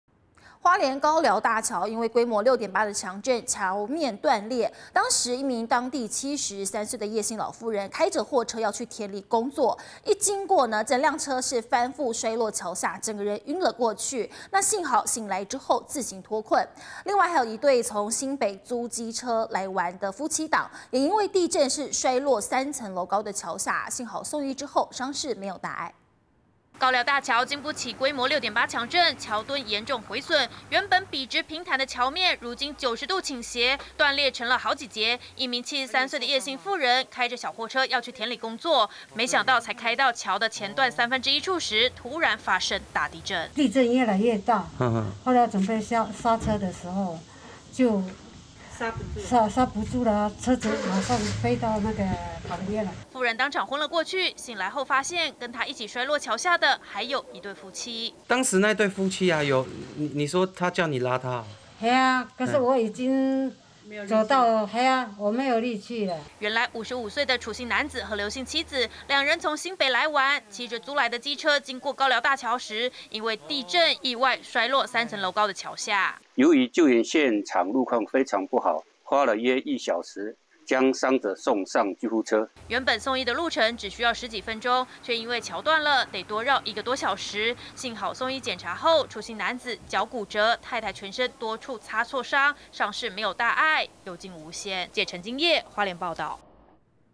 強震重創高寮橋 老婦開車行經翻覆慘摔橋下
花蓮高寮大橋，因為規模6.8強震，橋面斷裂，當時一名當地73歲的葉姓老婦人，開著貨車要去田裡工作經過，整輛車翻覆摔落橋下，整個人暈了過去，醒來後幸好自行脫困！另外還有一對從新北租機車來玩的夫妻檔，也因為地震摔落3層樓高的橋下，幸好送醫後，傷勢沒有大礙。